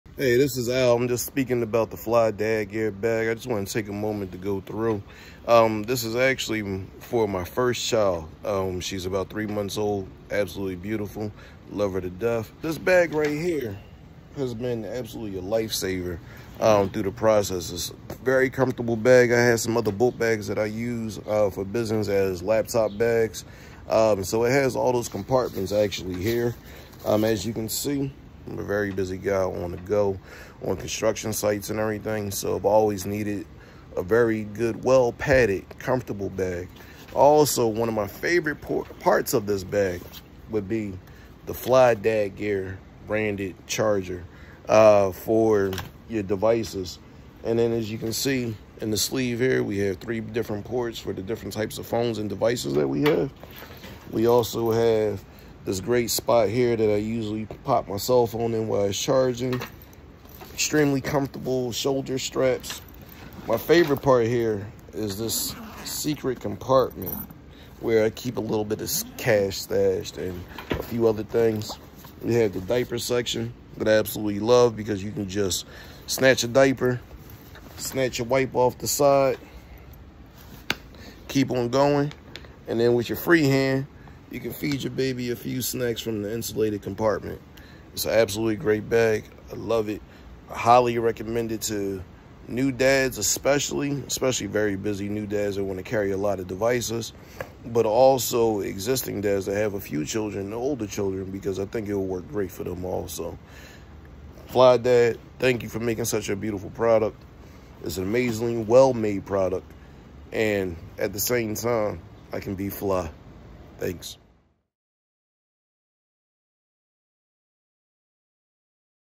First Born backpack testimonial
LISTEN UP, FOLKS!!!

The First Born backpack is designed for busy parents, especially for dads who need a perfect bag to fit their everyday essentials, whether for work or when going out with kids.

Check out this video from a real estate agent, Al,  and you'll know why the First Born backpack is a "life-saver" for all the dads. We got you covered!